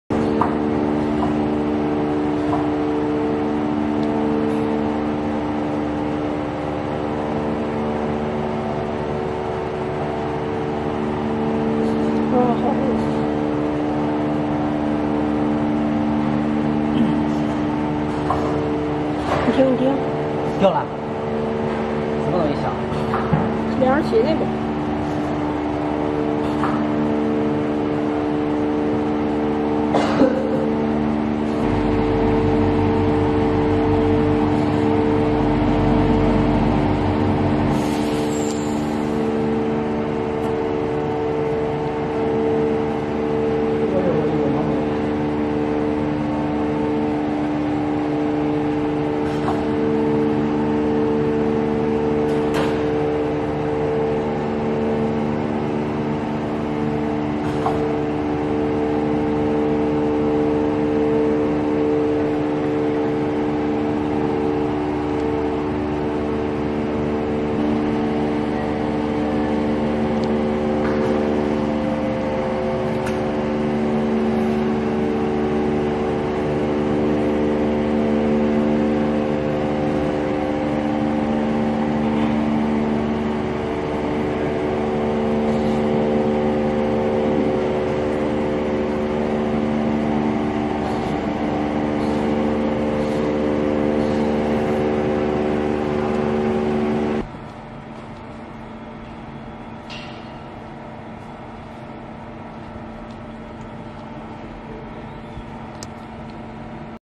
Newest ZrN Gold coating- Real-gold looklike finish
Royal technology has developed a newest highly-like real gold finish by Zr target. Which reduce the production cost and gets high aesthetic surface purpose.

Zirconium Nitride (ZrN) PVD coating has excellent functional properties except the aesthetic purpose.
It has much better corrosion resistance than TiN; also higher hardness, toughness, good wear resistance and biocompatible which applied with surgery tools, dental instruments, kitchenware etc.